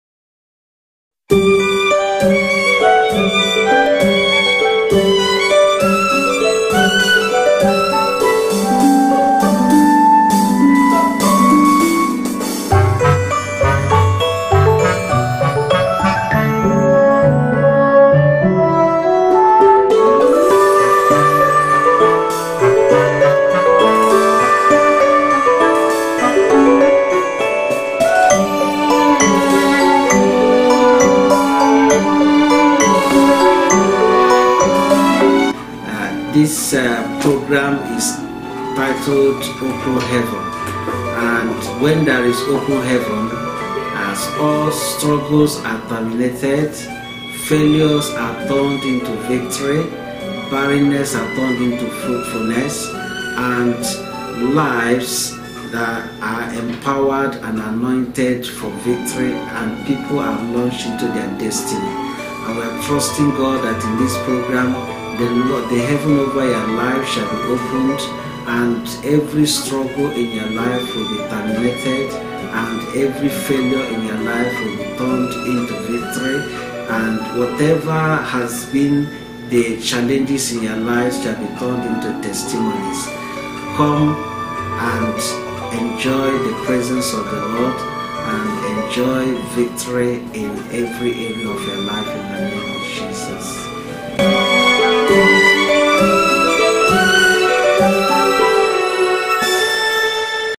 DIVINE ENCOUNTER PROMO/PASTOR'S MESSAGE
Come and be part of this great event and receive abundant blessings
from the Lord Most High.
Friday August 31- Sunday September 2, 2018. (DIVINE ENCOUNTER. Theme 'OPEN HEAVENS'.
GOD BLESS YOU AS YOU COME.